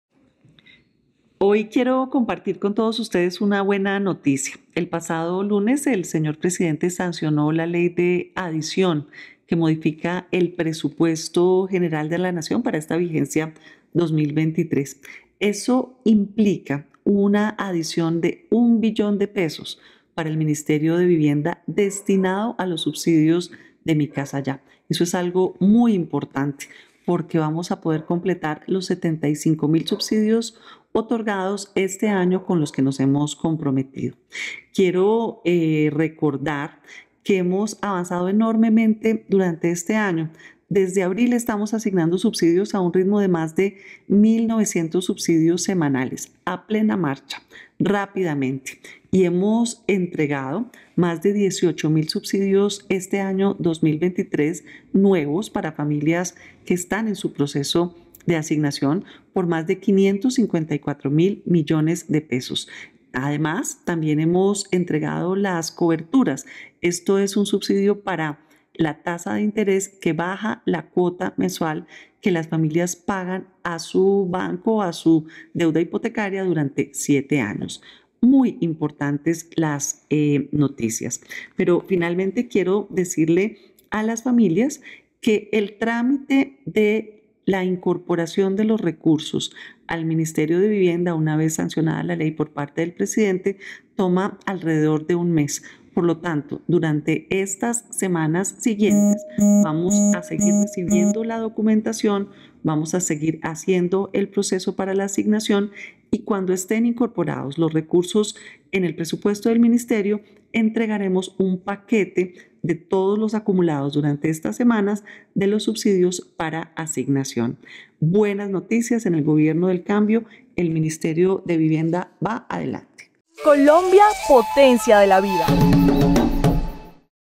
Fue sancionada la ley que permitirá otorgar 75.000 subsidios de vivienda en el 2023
En el marco de esta adición presupuestal fue aprobado $1 billón, con el que el Ministerio de Vivienda, Ciudad y Territorio cumplirá la meta de otorgar 75.000 subsidios este año.

La ministra de Vivienda, Catalina Velasco Campuzano, informó que mientras se surte el trámite legal de la adición, que se estima sea en un mes, el Ministerio continuará haciendo el proceso normal de recepción y revisión de documentación de hogares interesados en aplicar a las nuevas asignaciones, que se harán inmediatamente ingresen los recursos a la entidad.